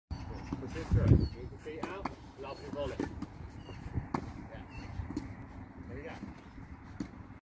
Volley training
Loads of Volley training which I love